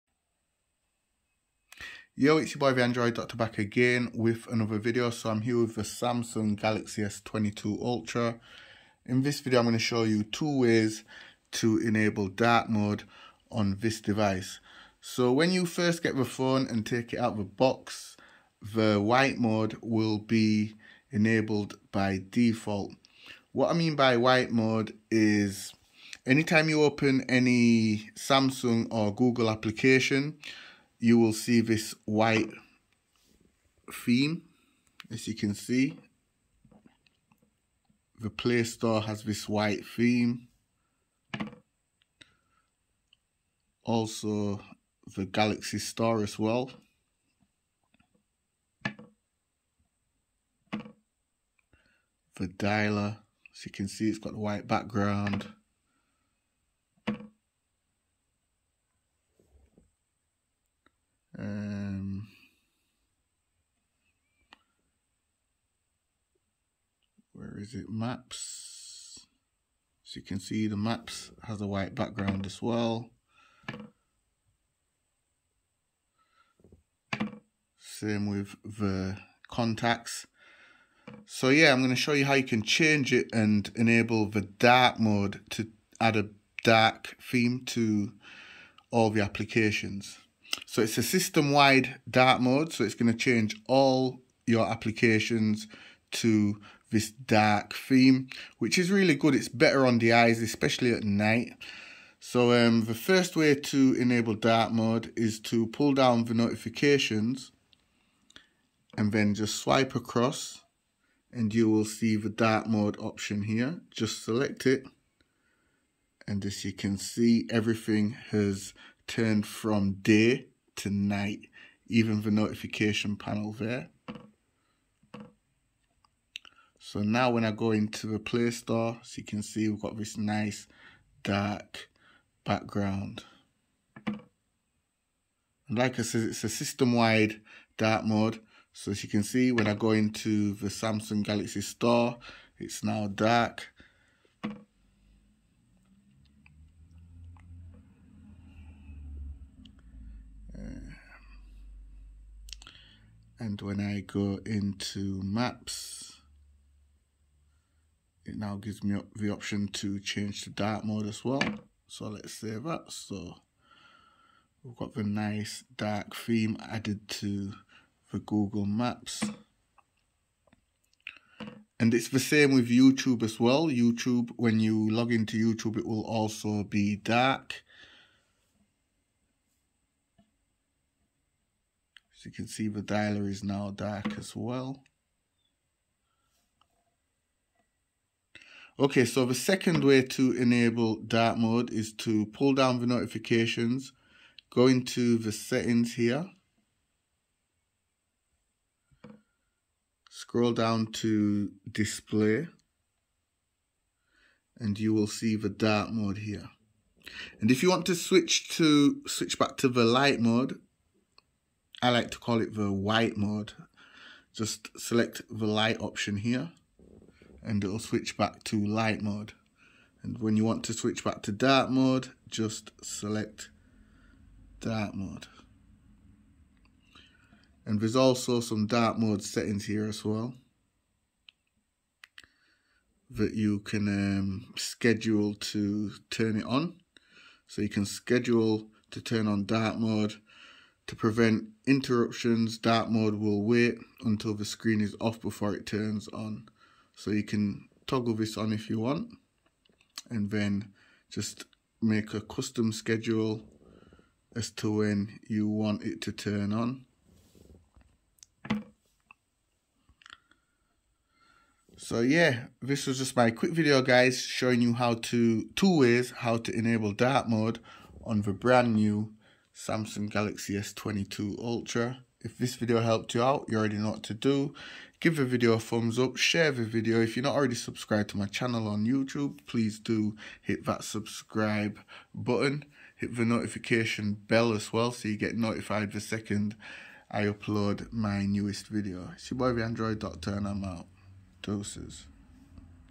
Samsung Galaxy S22 Ultra 2 Ways To Turn On The System Wide DARK-MODE Turn All Applications DARK!!
HEY EVERYONE ANDROID DOCTOR HERE AGAIN in todays video i will be sharing with you 2 Ways To Turn On The System Wide DARK-MODE To Turn All Your Applications DARK On The Samsung Galaxy S22 Ultra!!!